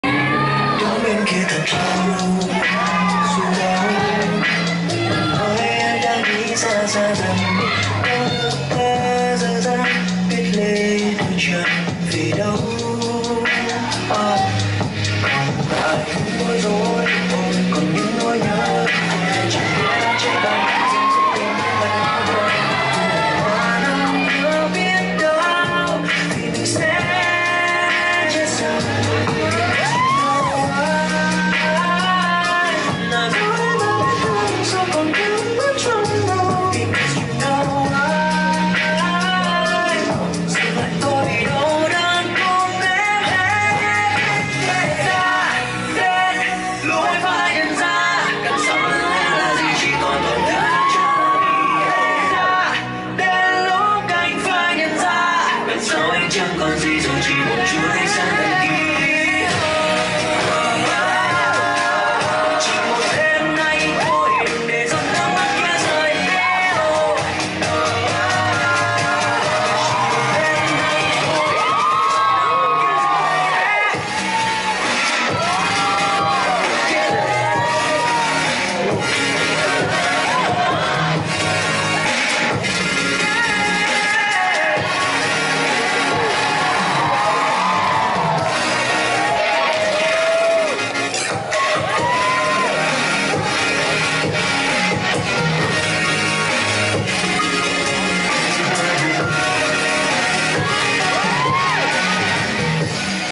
Múa quạt video 2 - Đêm Gia Đình (Family Night) 45 - Hội Học Sinh Việt Nam Trường Đại Học UMCP
Múa quạt - Traditional Fan Dance Video Clip Two in Family Night 45 by Vietnamese Student Association of University of Maryland at College Park on Saturday,  April 1, 2023 . Múa nón video clip 2 trong Đêm Gia Đình (Family Night) 45  - Hội Học Sinh Việt Nam tại Trường Đại học UMCP.